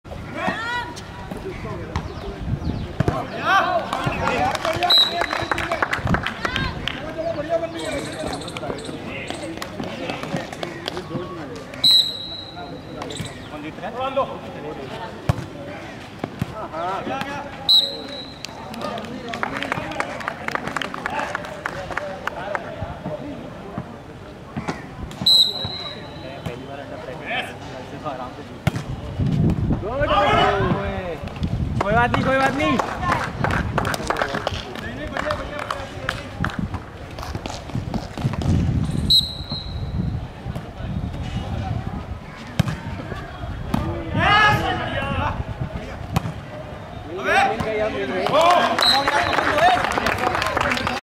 MARKIT VOLLEYBALL TOURNAMENT
We have organised an Intra Company volleyball tournament for the employees of Markit India Pvt. Ltd. Noida. In which 14 Teams Participated in the event with a gathering of around 150 people. The winner of the tournament was GPS INSTRUMENT, runner-up was WSO WARRIORS and second runner-up was MARKIT RDX